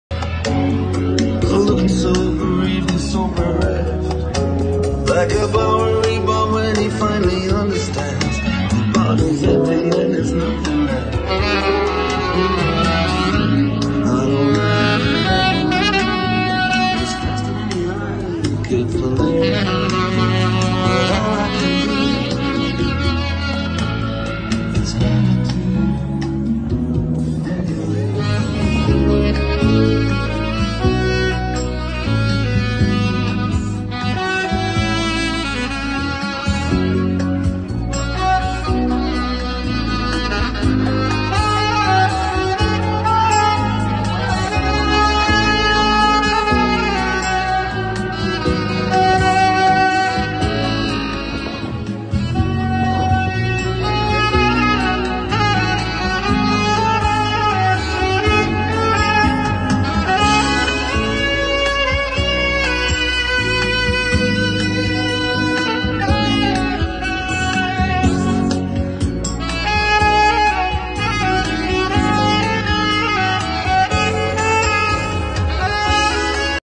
Abi Plays Sax // Live Footage // Book at Warble Entertainment
Abi Plays Sax is an international solo saxophonist and recording artist for high end corporate events and parties worldwide.